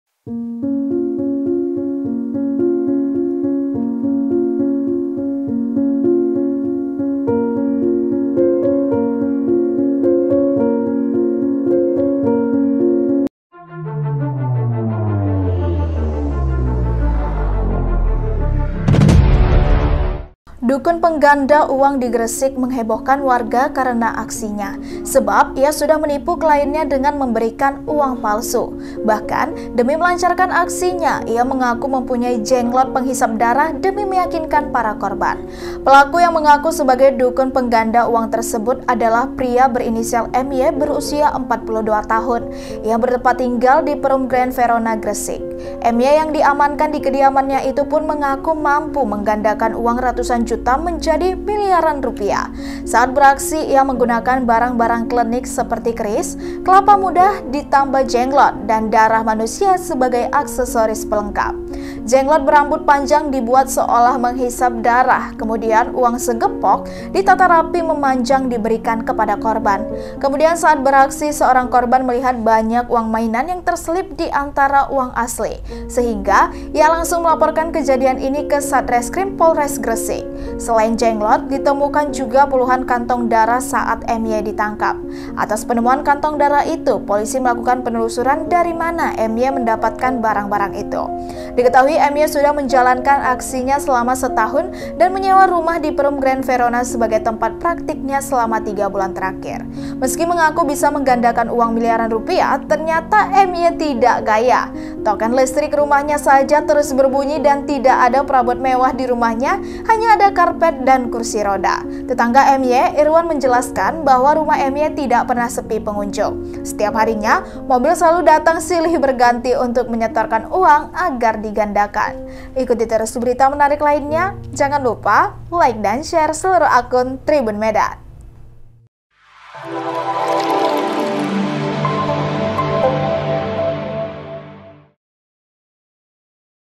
Dukun Pengganda Uang Ditangkap Polisi, Ngaku Punya Jenglot, Ketahuan Setelah Terselip Uang Palsu
TRIBUN-MEDAN.COM - Dukun pengganda uang di Gresik menghebohkan warga karena aksinya.

Sebab, ia sudah menipu kliennya dengan memberikan uang palsu.

Bahkan, demi melancarkan aksinya, ia mengaku mempunyai jenglot penghisap darah demi meyakinan para korban.

Pelaku yang mengaku sebagai dukun pengganda uang tersebut adalah pria berinisial MY (42).

Ia bertempat tinggal di Perum Grand Verona, Gresik.

MY yang diamankan di kediamannya itu mengaku mampu menggandakan uang ratusan juta menjadi miliar rupiah.

Saat beraksi ia menggunakan barang-barang klenik seperti keris, kelapa muda ditambah jenglot dan darah manusia sebagai aksesoris pelengkap.

Jenglot berambut panjang dibuat seolah menghisap darah kemudian uang segepok ditata rapi memanjang diberikan kepada korban.

Kemudian saat beraksi, seorang korban melihat banyak uang mainan yang terselip di antara uang asli sehingga ia langsung melaporkan kejadian ini ke Satreskrim Polres Gresik.